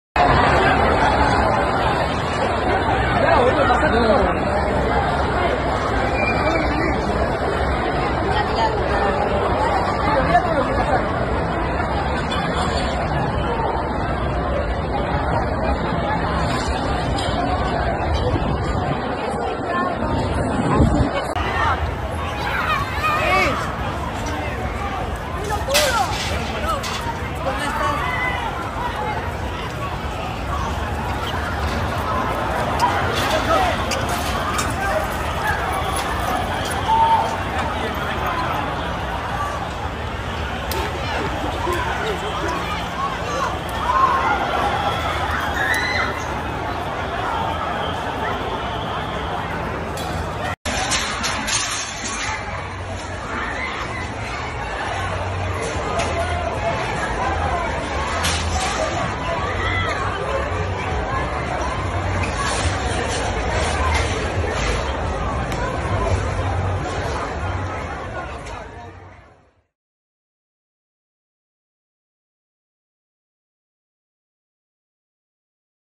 Final de la Copa América se retrasó por desmanes | El Espectador
La fiesta de la gran final entre Colombia vs. Argentina se retrasó media hora por algunos hinchas de la tricolor, quienes intentaron ingresar sin boleta al Hard Rock Stadium de Miami y llevaron a que cerraran las puertas de ingreso al estadio.